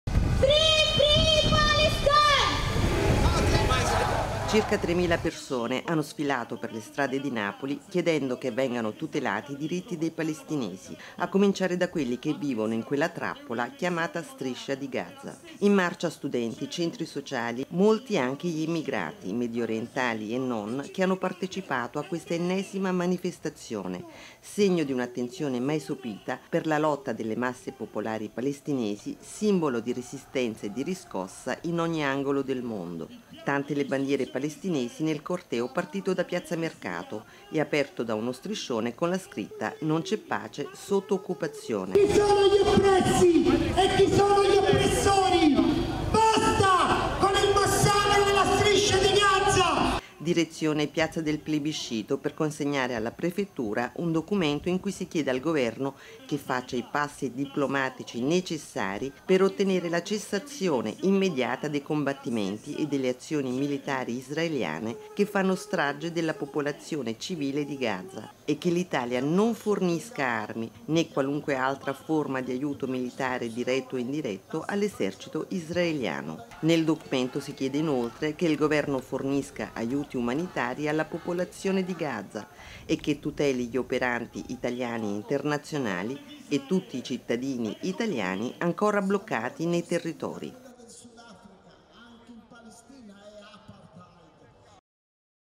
Guerra in Medioriente, migliaia in piazza a Napoli pro -Palestina
In migliaia hanno manifestato ieri a Napoli per sensibilizzare sulla guerra in Medioriente. “L’Italia sia neutrale” consegnato un documento al rappresentante di governo dagli attivisti per la libertà della Palestina